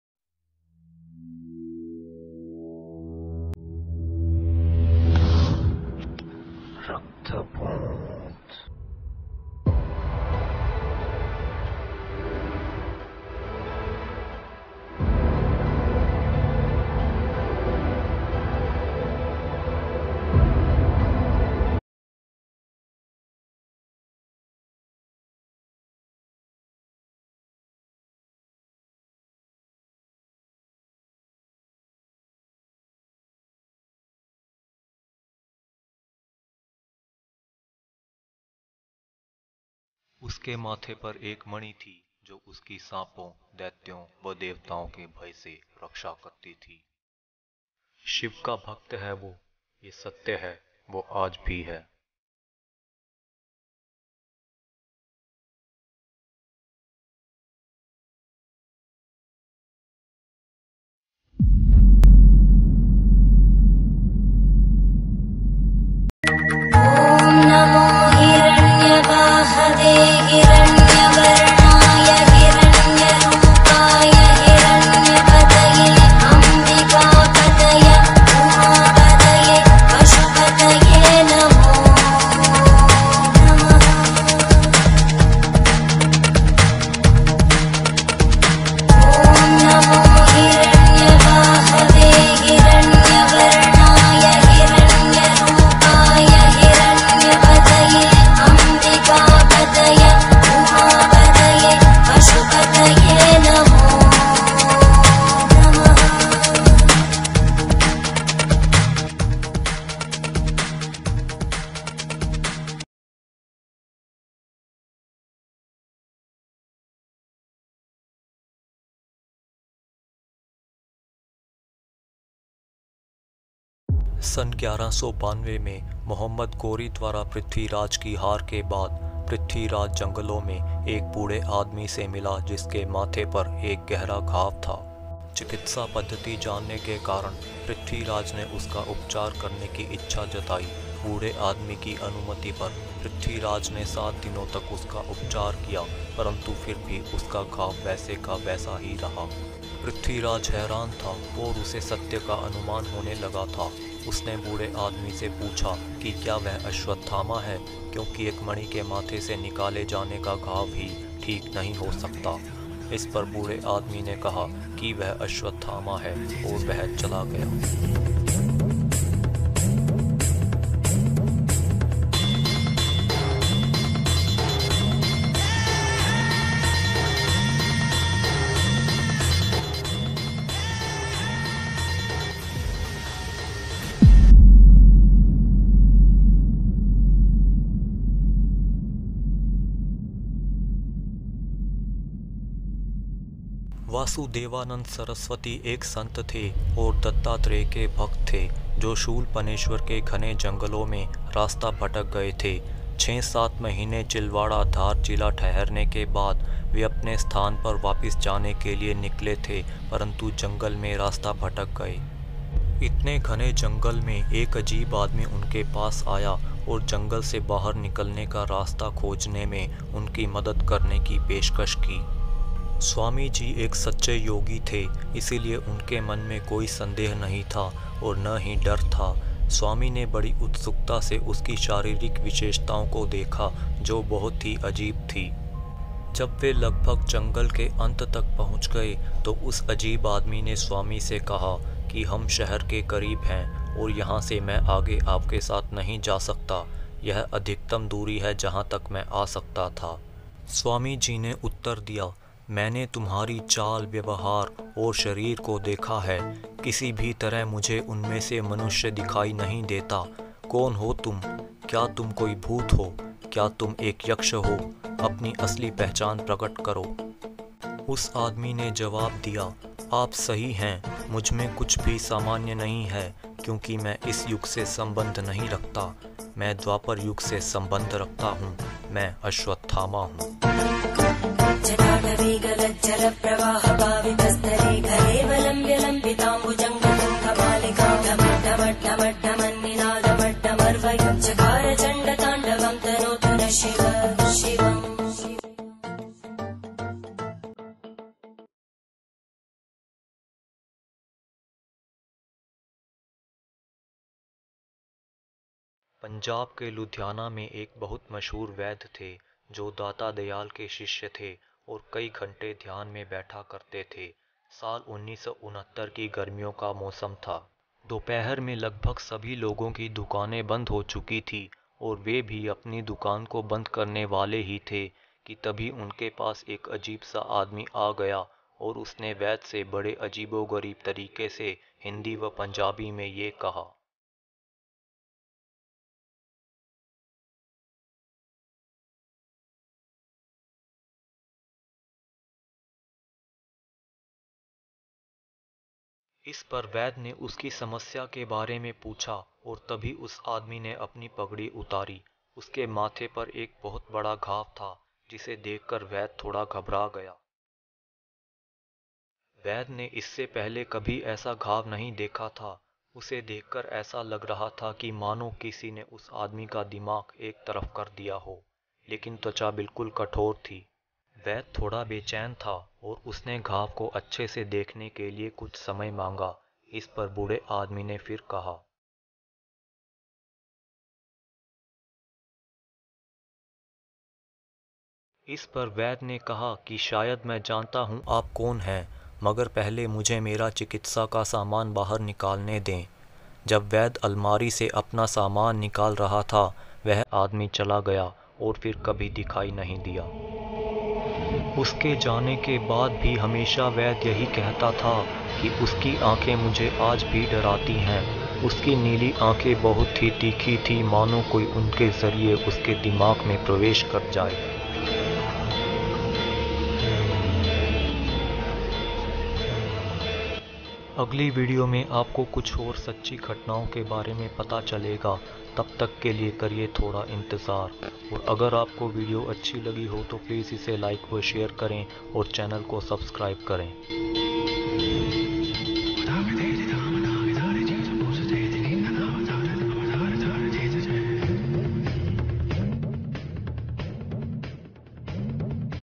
अश्वथामा से जुड़ी लोकप्रचलित कहानियां(part 1)you will be shocked /Ashwathama 5000years
(captions are available)
"Ashwathama" 5,000 years old man
कृपया वीडियो को इयरफोन लगाकर सुने ताकि आपको यह अच्छी तरह से समझ आ सके।
अश्वथामा को कलयुग के अंत तक भटकते रहने का श्राप मिला है।कोई संत हो या अवतार,वह चाहे वरदान दे या श्राप हमेशा सिद्ध होता है।